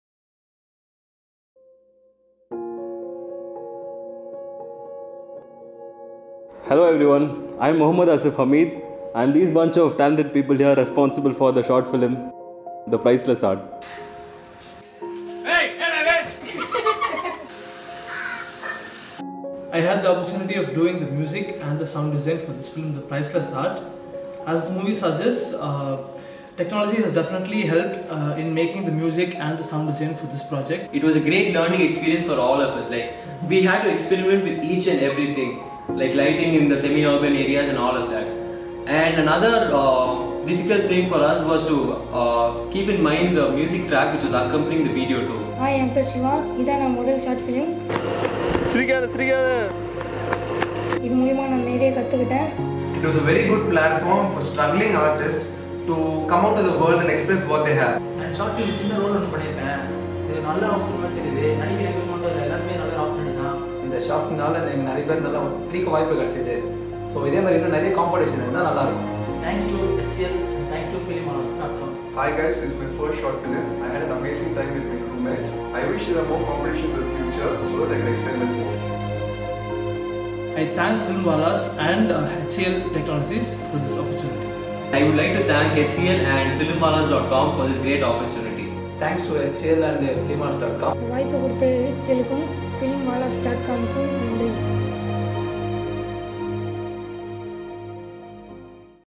The Priceless Art I Behind the Scenes I Filmwallas.com
The Cast and Crew of the ACutBeyond contest prize-winning film, The Priceless Art, share their thoughts on their filmmaking process with Filmwallas.com.